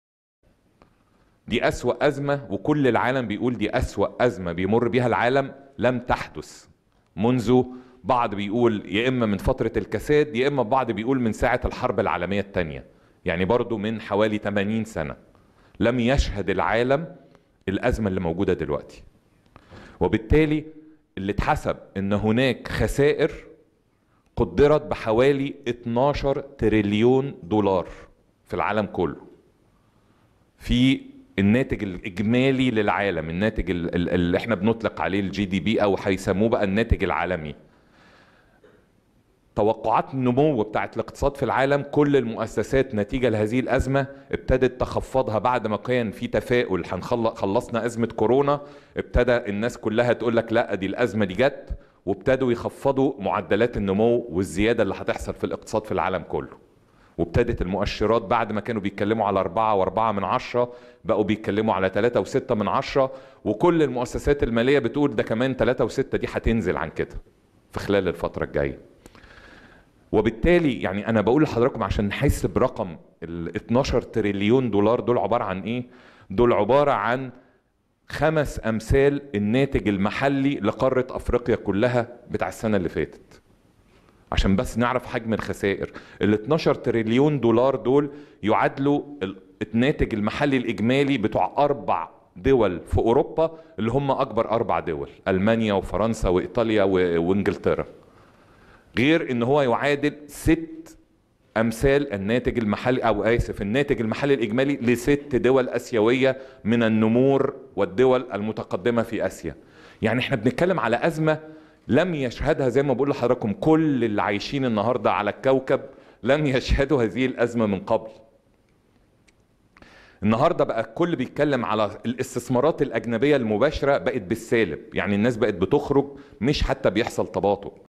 رئيس الوزراء: لم يشهد العالم مثل هذه الأزمة منذ الحرب العالمية الثانية وهناك خسائر قدرت بـ 12 ترليون
قال الدكتور مصطفى مدبولي رئيس الوزراء أن العالم لم يشهد مثل هذه الأزمة منذ الحرب العالمية الثانية وهناك خسائر قدرت بـ 12 ترليونجاء ذلك خلال المؤتمر الصحفي العالمي للإعلان عن رؤية الدولة للتعامل مع الأزمة_الاقتصادية العالمية الحالية، وكذا خطط وجهود جذب الاستثمارات المحلية والأجنبية، و ويأتى ذلك تنفيذا لتوجيهات الرئيس عبد الفتاح السيسي رئيس الجمهورية